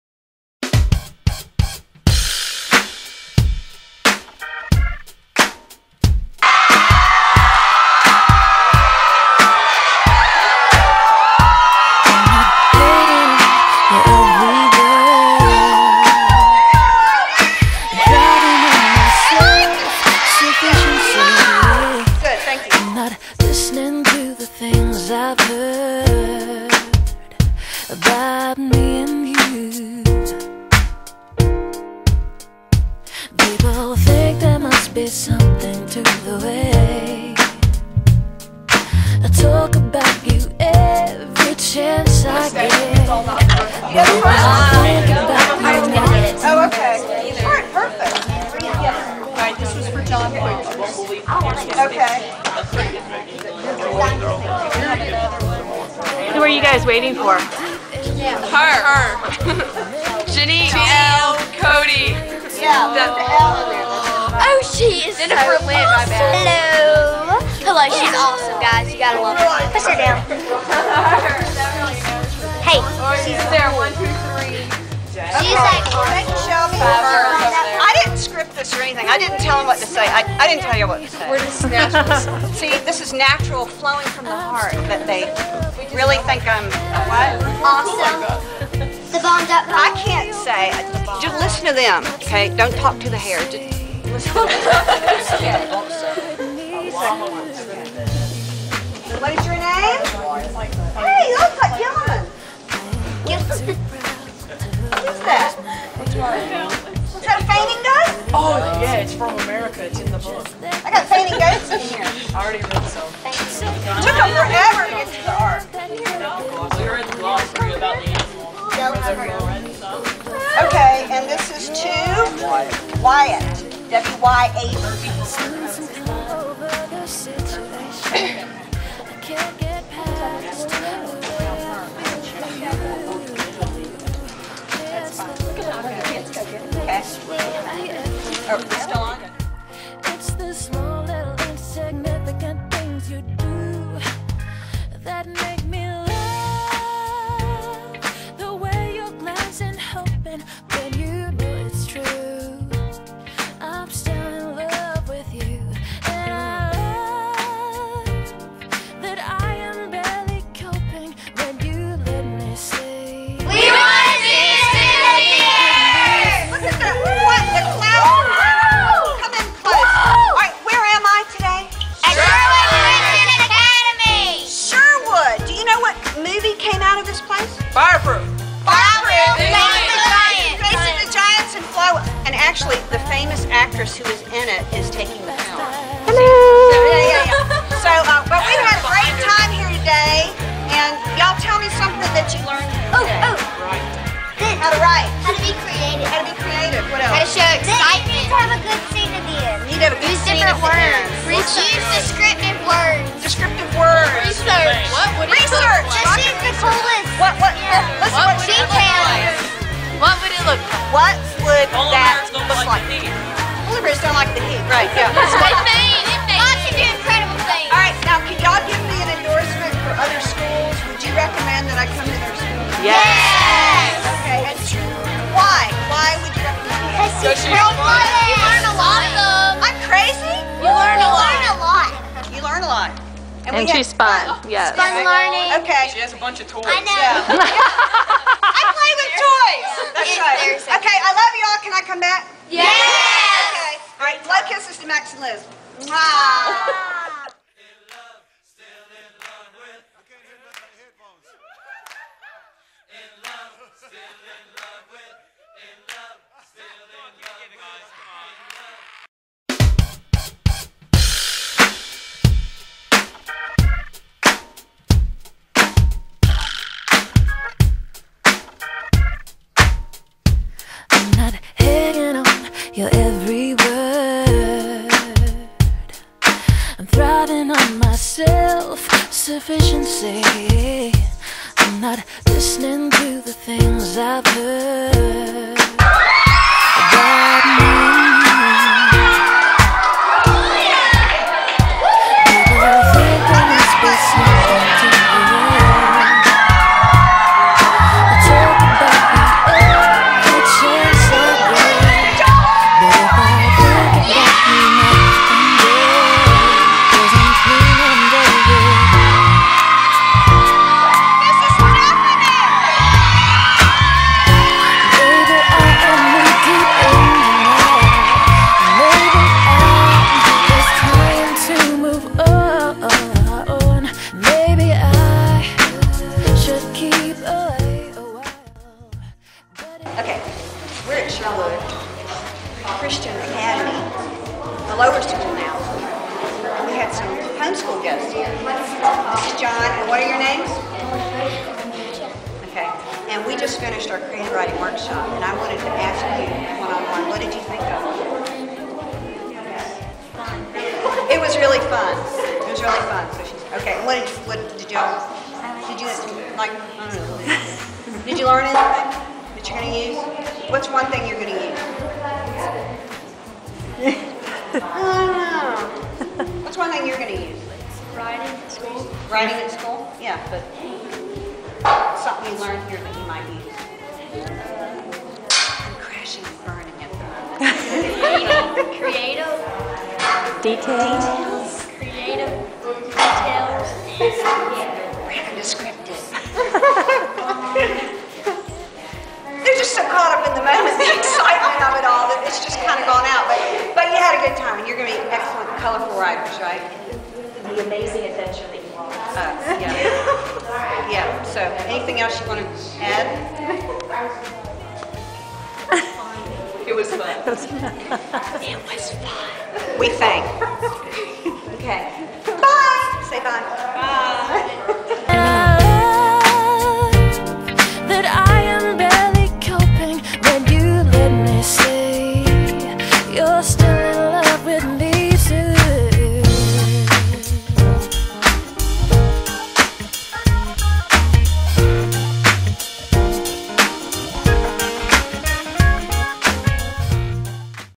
Jenny L. Cote visits Sherwood Christian Academy Albany GA October 2009.wmv
Jenny spent the entire day with the Middle and Lower Schools of Sherwood Christian Academy!